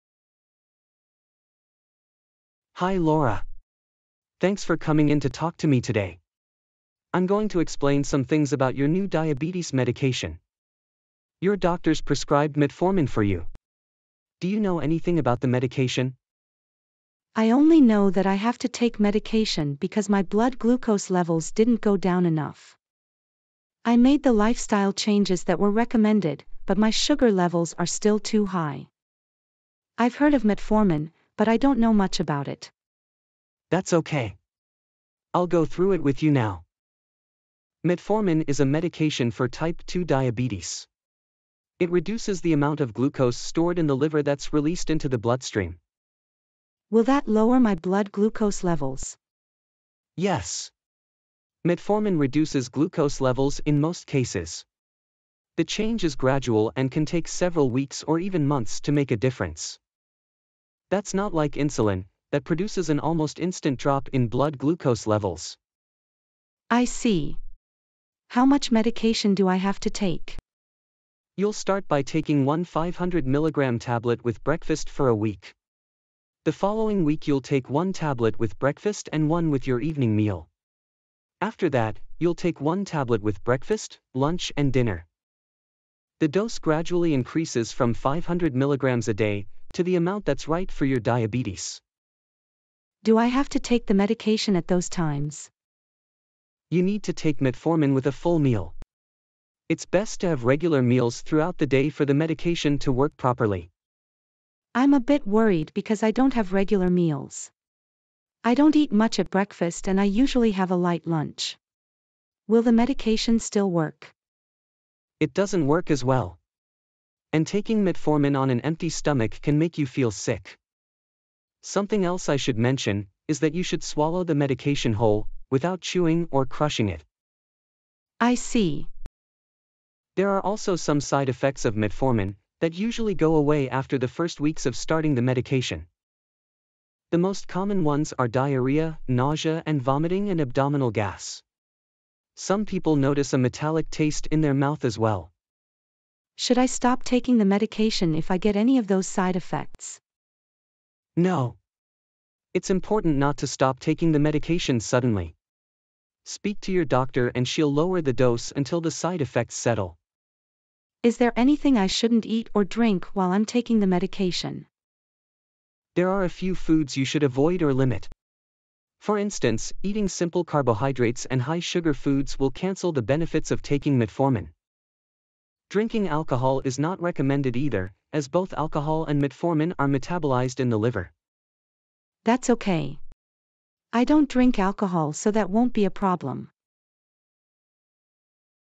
English for Nurses: Explaining Metformin for Type 2 Diabetes (US Version)
US VERSION: Learn how to explain a new prescription of Metformin to a patient (dose, side effects, precautions).